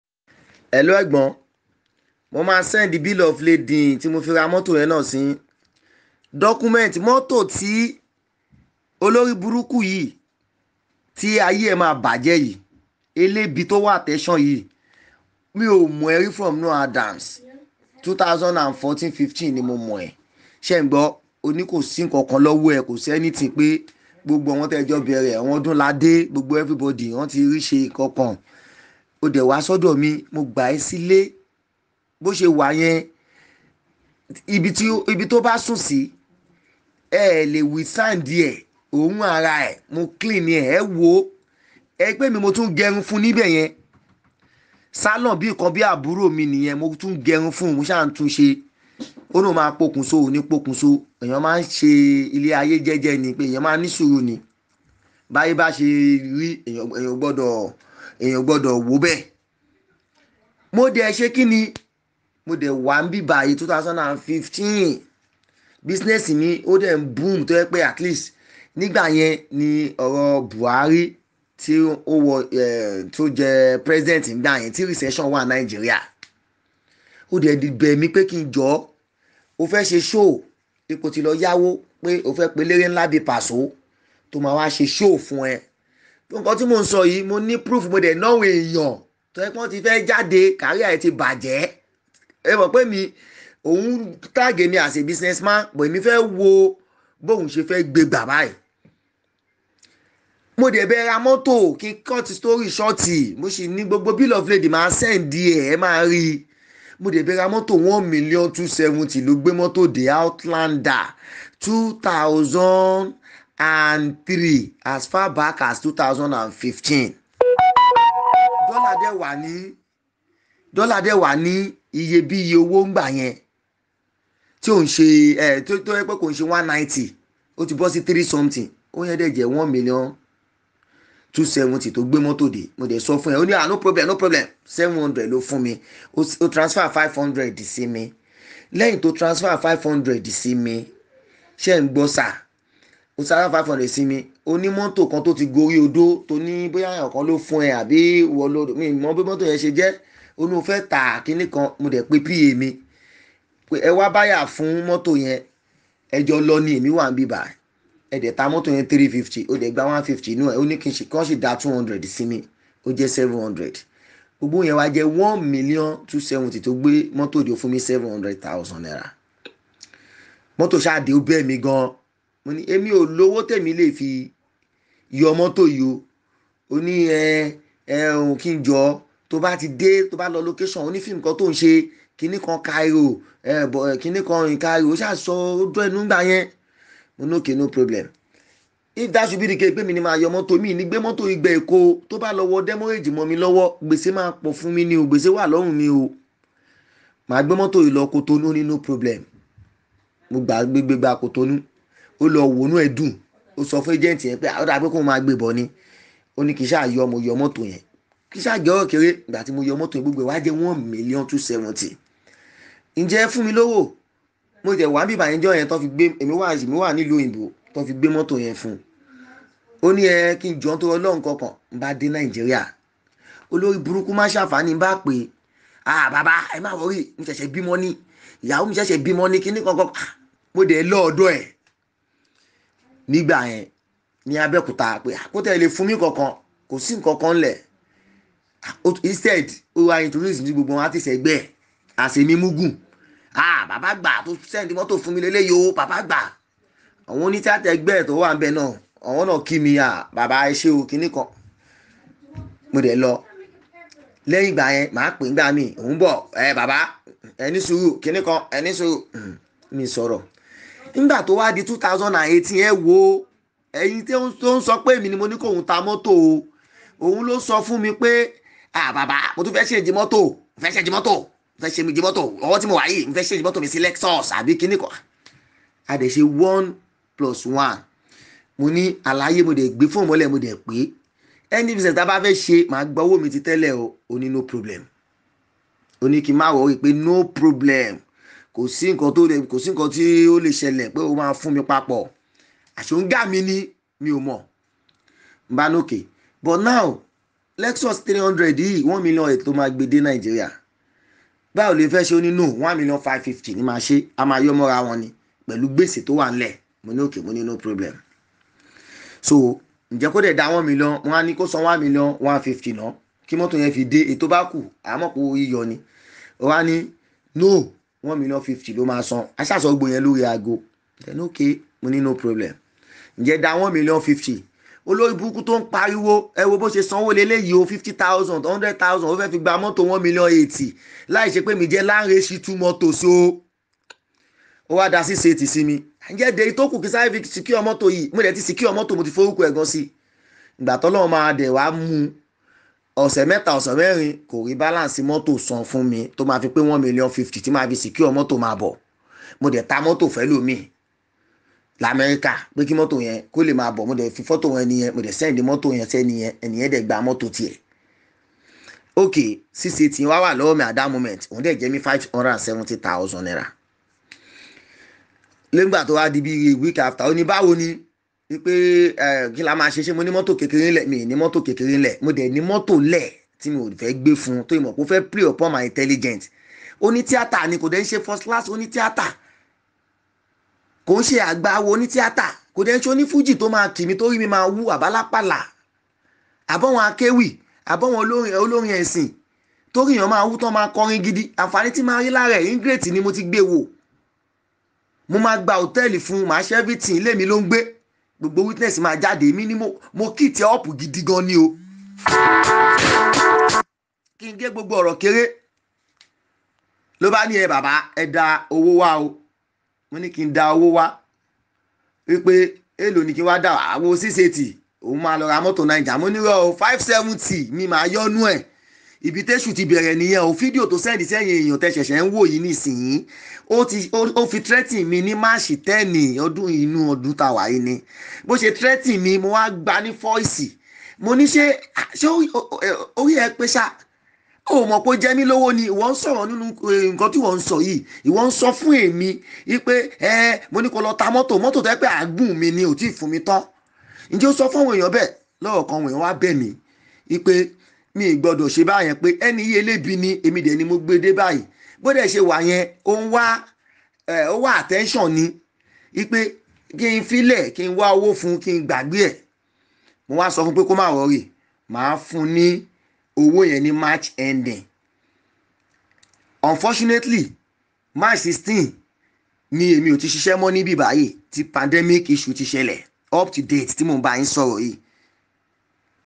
Ismaila Afolabi Replies Nollywood Actor: "I'm Not Owing Him, He's an Ingrate Seeking Attention"
In an audio and screeshots Mr Ismaila Afolabi shared with Ayaba Kadejo, he explained his relationship with the Nollywood Actor Olu Olowogemo popularly known as Arijan Mr Portable and what actually transpired.